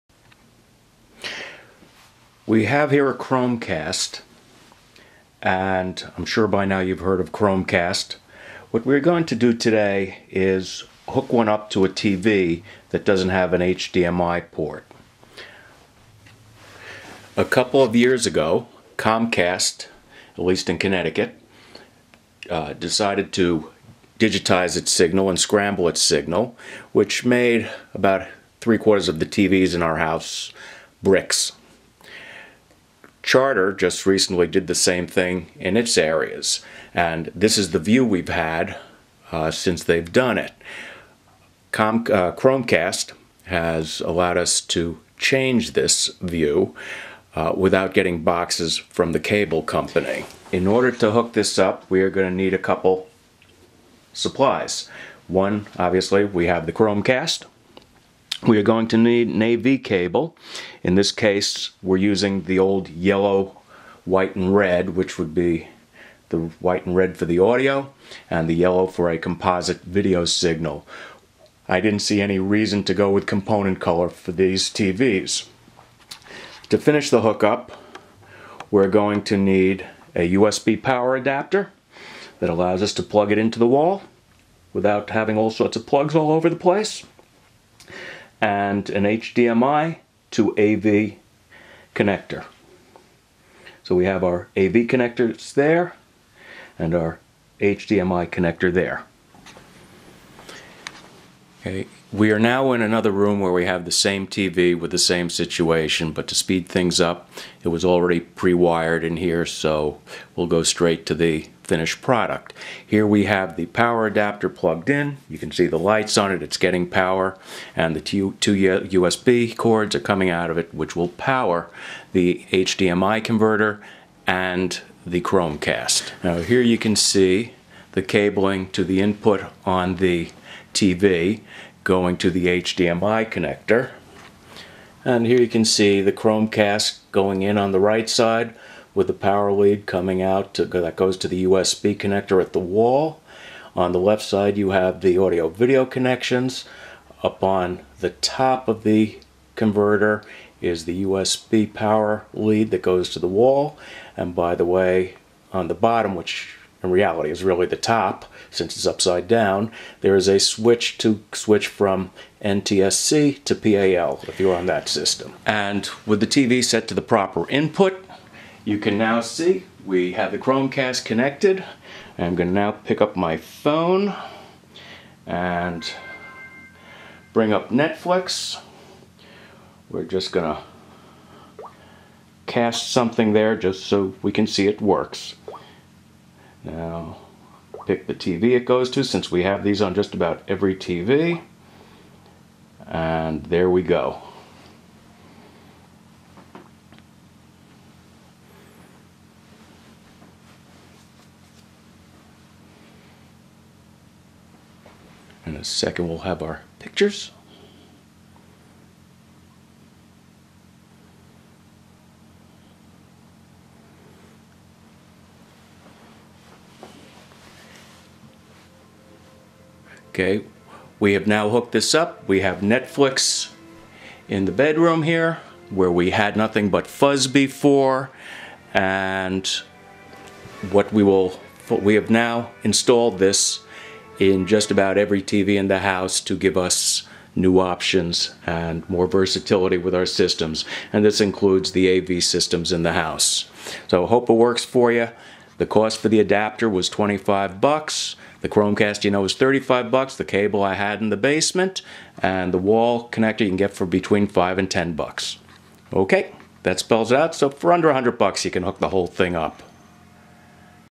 Chromecast to AV on an older TV with no HDMI
Chromecast comes with an HDMI connector. This video shows how to connect it to an older TV that does not have an HDMI port. I will be connecting the Chromecast to a Composite color port on a TV. This can also be done with Component Color as well using a different adapter, at a slightly higher price.